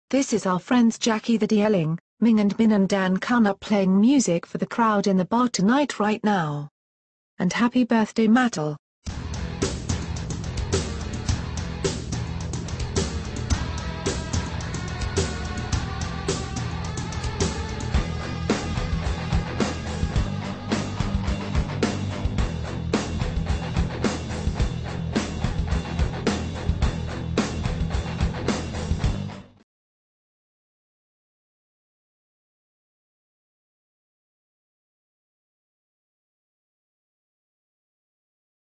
Rocking on at the bar!
Jackie deerling, ming, minne and dan-kun are having fun in a bar!